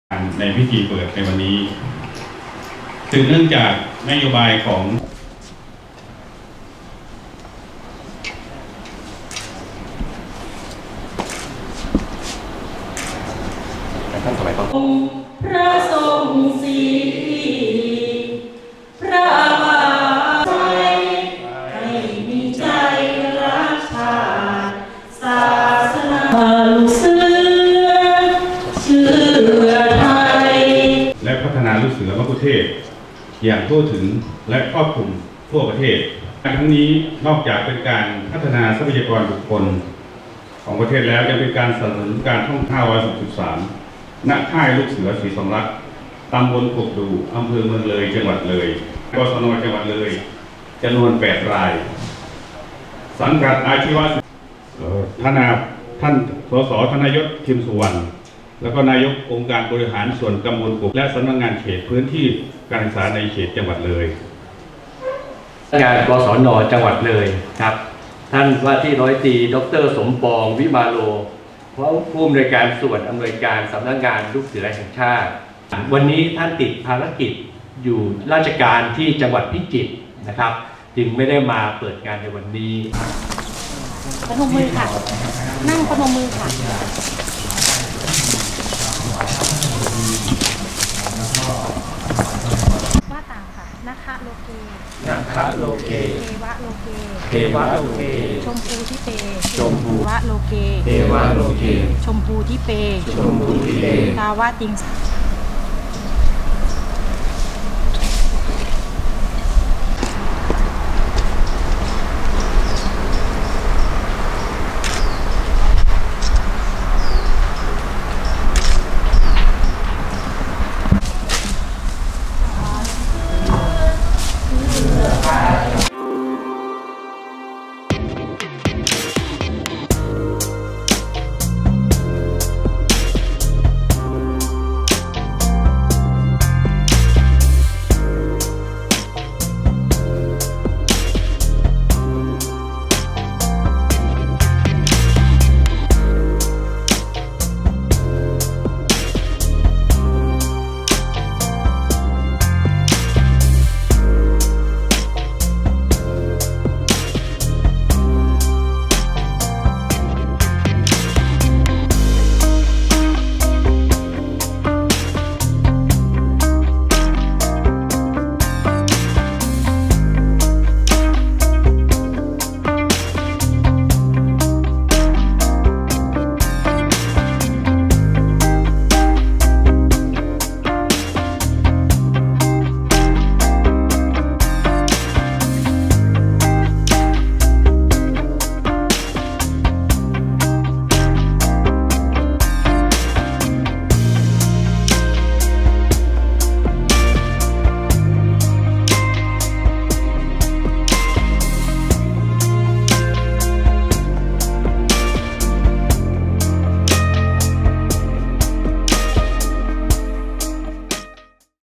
เลยนำร่อง 8 จังหวัด ปั้นลูกเสือเป็นไกด์นำเที่ยว
ที่ค่ายลูกเสือศรีสองรัก  อ.เมือง  จ.เลย  เมื่อวันที่ 6 มกราคม 2563  นายสมเกียรติ  ตันดิลกตระกูล  ผู้ช่วยเลขานุการรัฐมนตรีช่วยว่าการกระทรวงศึกษาธิการ  เป็นประธานพิธีเปิดโครงการอบรมลูกเสือมัคคุเทศก์  โดยมีผู้บริหารสถานศึกษา หน่วยงานด้านการศึกษาในจังหวัดเลย ให้การต้อนรับและร่วมพิธี

ว่าที่ ร.ท.จำนงค์  นนทะมาศ  รองผู้อำนวยการ กศน.จังหวัดเลย  กล่าวว่า  สืบเนื่องจากนโยบายของกระทรวงศึกษาธิการในการพัฒนาลูกเสือให้มีสมรรถนะด้านมัคคุเทศก์ น้อมนำหลักสูตรจิตอาสา 904 สร้างระเบียบวินัยและปลูกจิตอาสา เพื่อสืบสานพระราชดำริของพระบาทสมเด็จพระเจ้าอยู่หัวรัชกาลที่ 10 โดยความร่วมมือกับกระทรวงท่องเที่ยวและกีฬา ในการขับเคลื่อนพัฒนาลูกเสือมัคคุเทศก์อย่างทั่วถึง ให้ครอบคลุมทุกจังหวัด โดยเริ่มนำร่อง 8 จังหวัด และขยายผลต่อไปอีก 69 จังหวัด

การดำเนินโครงการนี้  นอกจากเป็นการพัฒนาทรัพยากรบุคคลของประเทศแล้ว ยังเป็นการสนับสนุนการท่องเที่ยวของประเทศอีกด้วย ในการนี้สำนักงานลูกเสือแห่งชาติร่วมกับจังหวัดเลย  จึงได้กำหนดจัดฝึกอบรมลูกเสือมัคคุเทศก์ขึ้นระหว่างวันที่ 6-9 มกราคม 2563  โดยมีผู้เข้าร่วมการฝึกอบรมประกอบด้วยลูกเสือ และเนตรนารีจากสังกัดสำนักงานกศน.จังหวัดเลย , สังกัดอาชีวศึกษาเลย  , สังกัดสำนักงานเขตพื้นที่การศึกษาประถมศึกษาเลยเขต 1 , สำนักงานเขตพื้นที่ประถมศึกษาเลยเขต 2 , สำนักงานเขตพื้นที่การศึกษาประถมศึกษาเลยเขต 3  , สังกัดสำนักงานเขตพื้นที่การศึกษามัธยมศึกษาเขต 19  , สังกัดสำนักงานคณะกรรมการส่งเสริมการศึกษาเอกชนจำนวน , สังกัดองค์กรปกครองส่วนท้องถิ่นจังหวัดเลย รวมทั้งสิ้น 60 คน

นายสมเกียรติ  ตันดิลกตระกูล  ผู้ช่วยเลขานุการรัฐมนตรีช่วยว่าการกระทรวงศึกษาธิการ  ปัจจุบันการท่องเที่ยวถือว่ามีความสำคัญมาก สามารถกระตุ้นเศรษฐกิจ นำรายได้เข้าสู่ประเทศชาติเป็นจำนวนมาก  ดร.กนกวรรณ วิลาวัลย์  รัฐมนตรีช่วยว่าการกระทรวงศึกษาธิการ  ที่ดูแลสำนักงานลูกเสือแห่งชาติ ได้ให้ความสำคัญกับเรื่องนี้เป็นอย่างมาก  โดยเฉพาะจังหวัดเลยมีแหล่งท่องเที่ยวที่มีชื่อเสียงเป็นอันดับต้นๆของประเทศ การเตรียมคนให้พร้อมต้อนรับผู้มาเยือน จึงต้องดำเนินการอย่างสม่ำเสมอ และมีประสิทธิภาพ ซึ่งทางกระทรวงศึกษาธิการได้ประสานกับกระทรวงท่องเที่ยวและกีฬา เพื่อให้การรับรองเป็นมัคคุเทศก์ที่ถูกต้องตามกฎหมาย  ลูกเสือและเนตรนารีที่ผ่านการอบรมไปแล้ว อาจจะต่อยอดไปสู่การประกอบอาชีพในอนาคตได้.